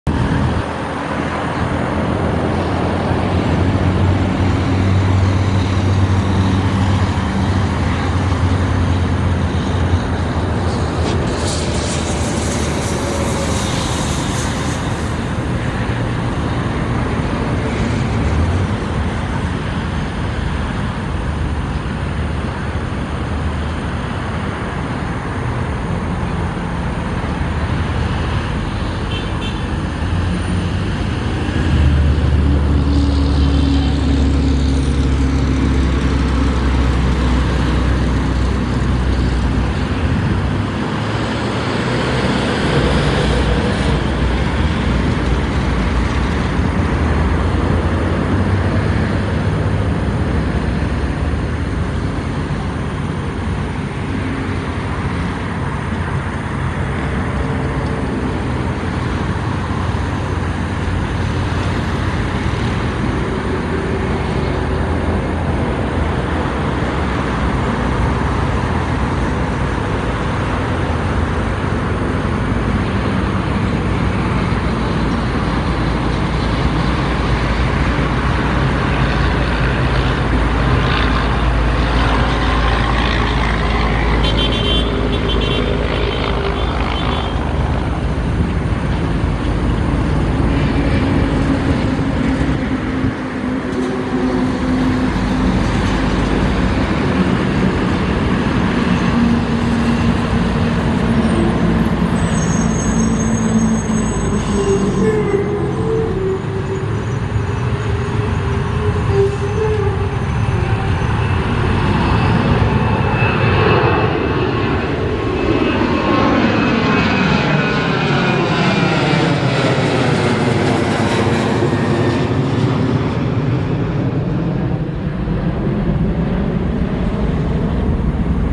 Wingo take off
Despegue Aeropuerto Internacional Juan Santamaria. SJO